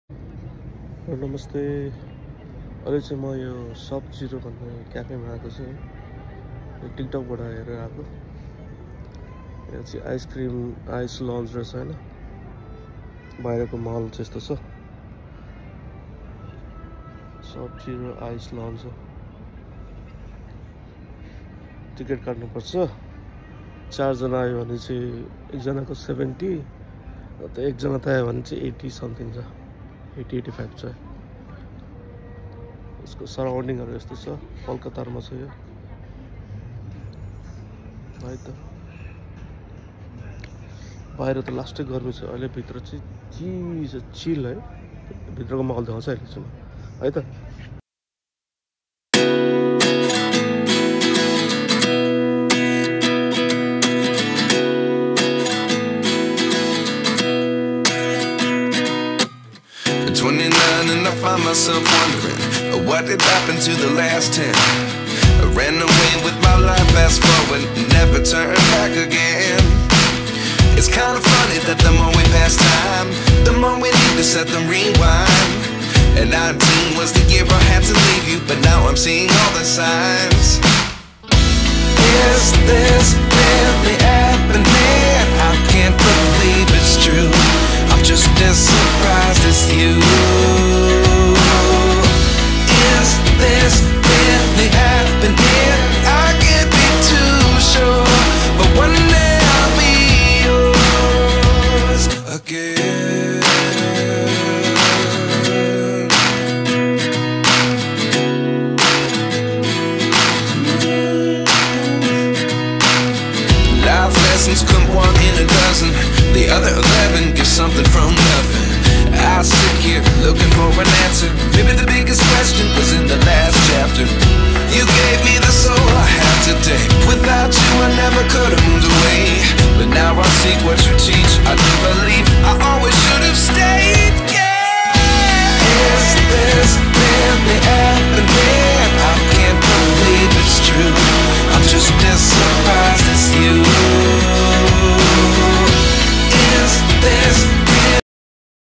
Qatar ma first time vlog gardai
Hi guys finally i got chance to visit Icelounge cafe in pearl qatar. Restaurant name Subzero . You can search in google and follow waze or google map to visit there. There was people coming after seeing TikTok and youtube vlogs. Even we also got to know from tiktok